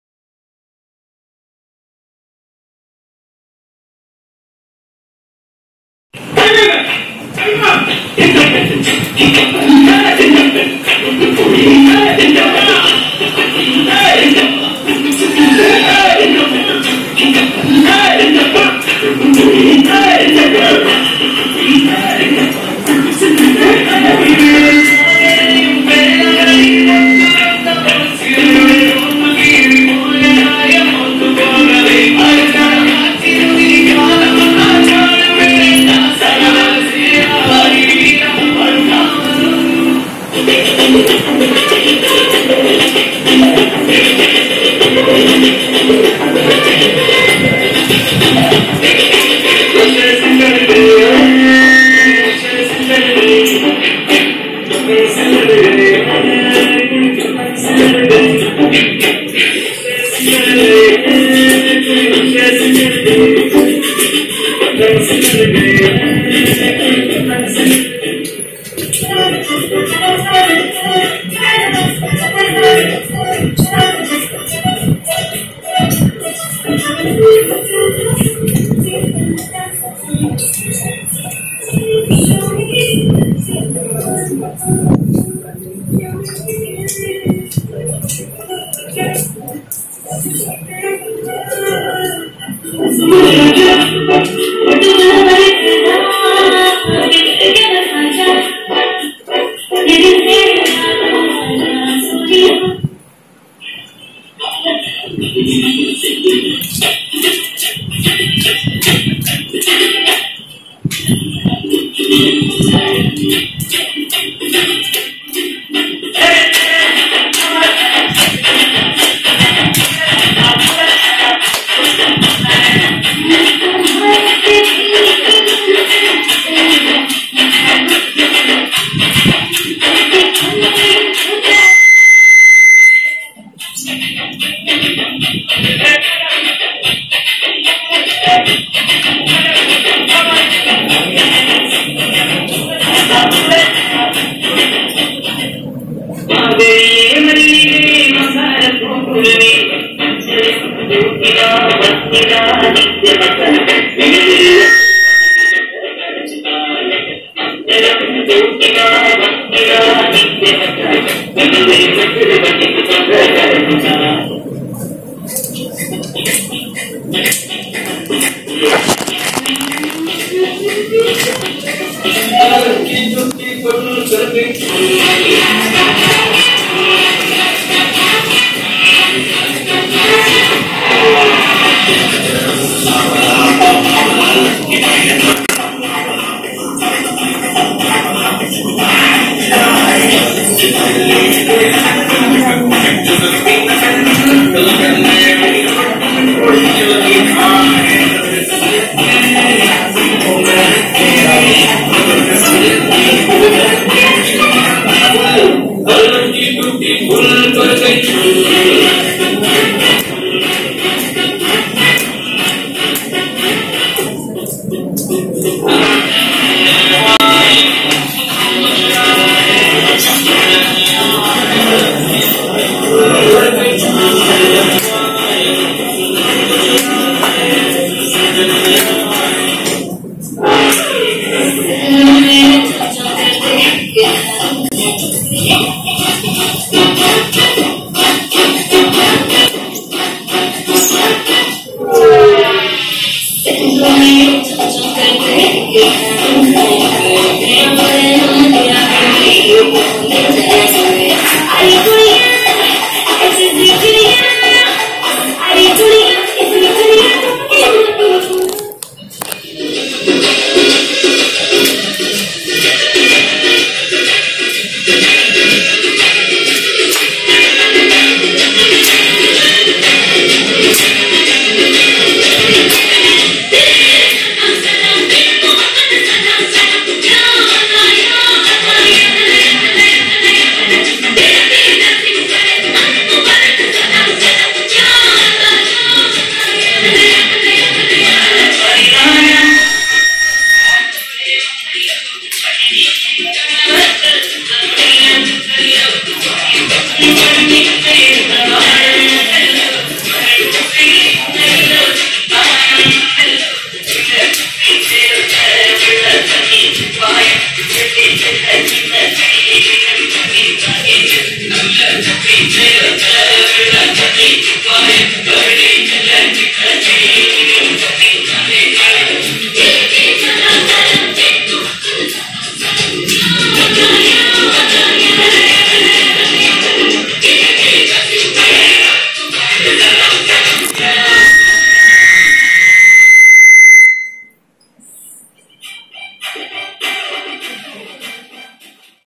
Super Mathematics Dance
A super Mathematics dance performance presented by the students of the Department of Computer Science, Central University, Kasargod